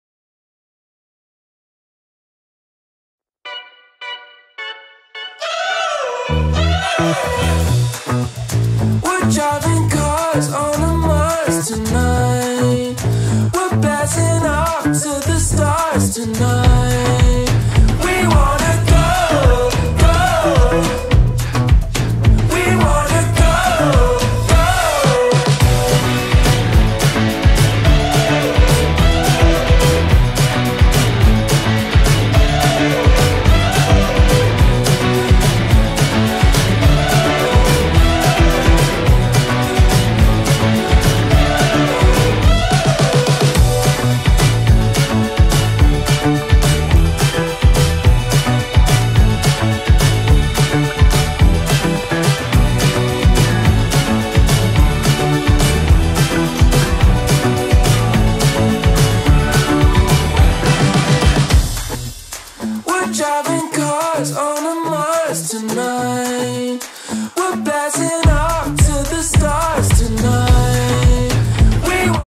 아이콘 김동혁, ‘깔끔한 셀럽 패션’(2023 S/S 서울패션위크)/ iKON DK(Donghyuk)
10월 11일 오후 서울 중구 동대문디자인플라자(DDP)에서 2023 S/S 서울패션위크(SFW) 송지오(SONGZIO) 포토월이 진행됐다.

현장에는 가수 아이콘 멤버 김동혁이 참석해 포토타임을 가졌다.